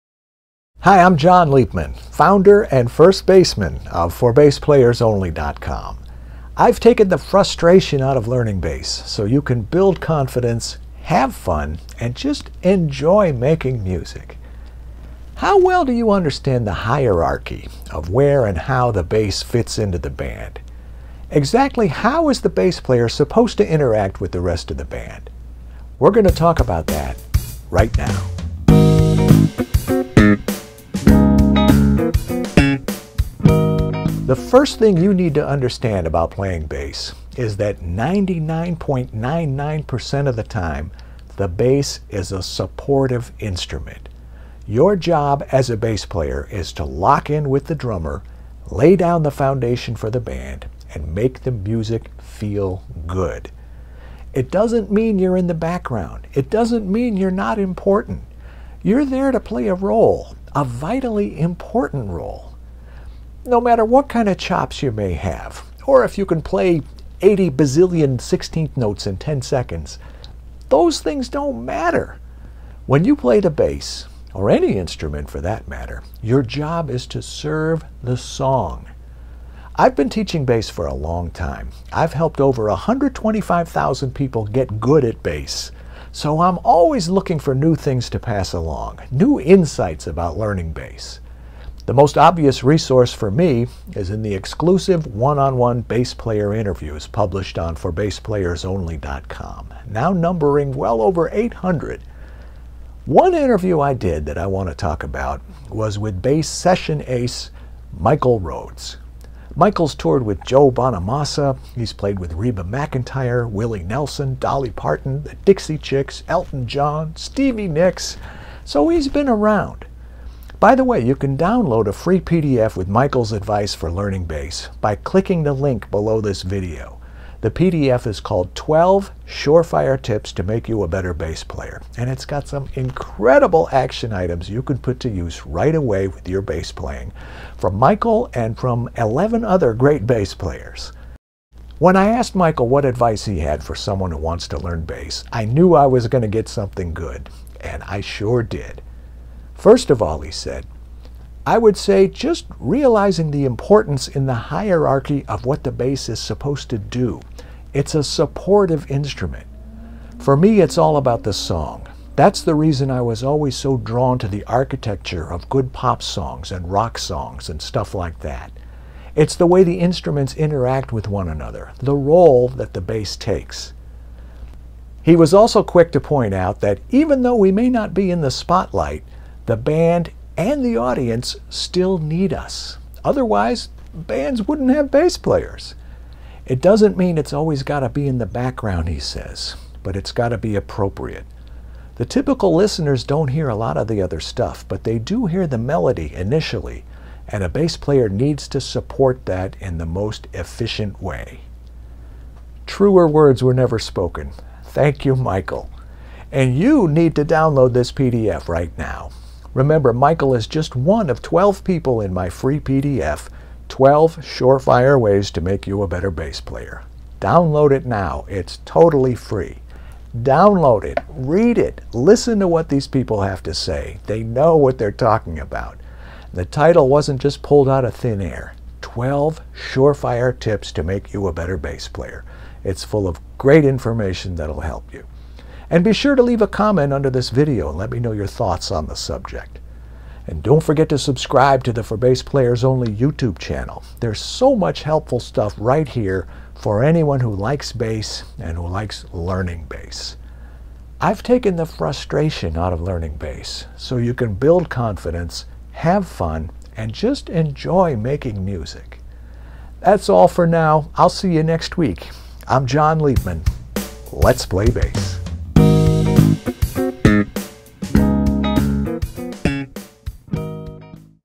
Understanding the hierarchy of the bass player in a band
How well do you understand the hierarchy of where the bass fits in with the rest of the band? If you think the bass is a "background" instrument... think again! Here's an easy-to-understand breakdown of how everyone in the band needs to work together, inspired by an interview with bass session ace Michael Rhodes!

FREE PDF!
12 SUREFIRE TIPS TO MAKE YOU A BETTER BASS PLAYER!
First-hand advice from Mike Merritt, Leland Sklar, Tony Levin, and 9 more!